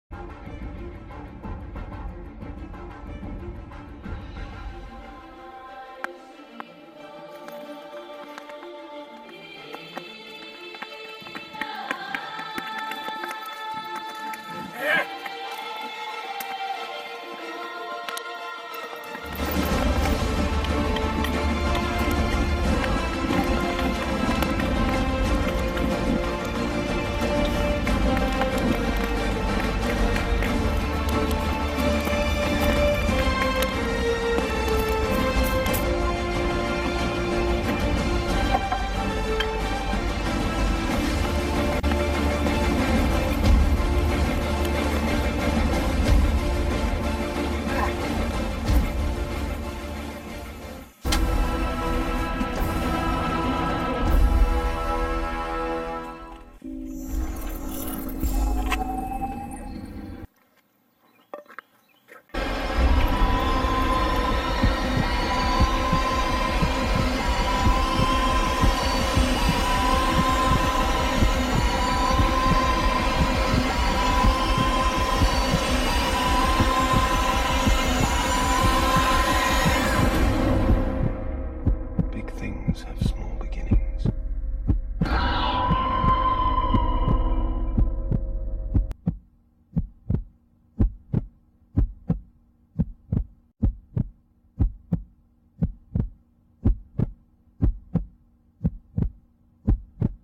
Hunger Games Backyard Battle (Trailer)
This is a Trailer for a short video were making. The trailer is not even fully done, with no special effects but, its the best i could do in 45 minutes with an Iphone 4s.